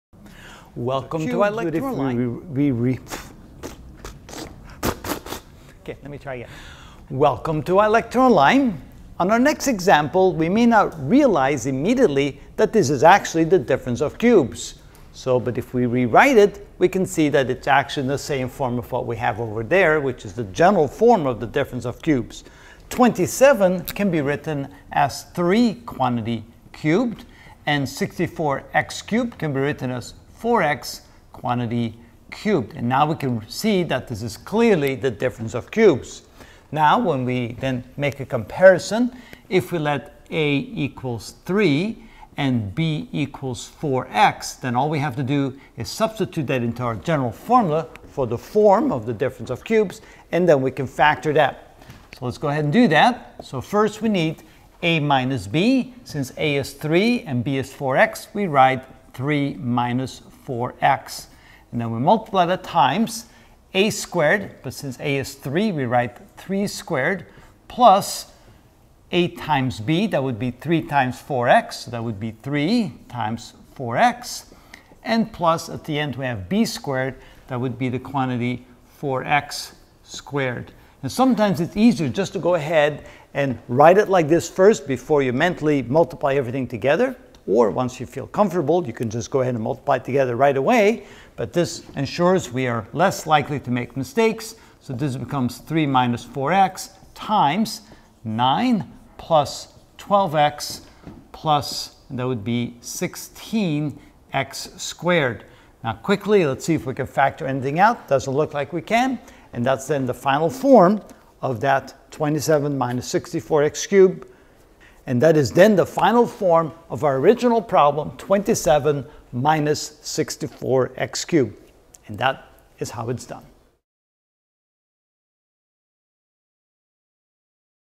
Algebra - Ch. 6: Factoring (54 of 55) Factoring Difference of Cubes.: Ex. 2/3
In this video I will show example 2 of 3 of factoring 27-64x^3 using the Difference of Cubes Method.

Next video in this series can be seen at: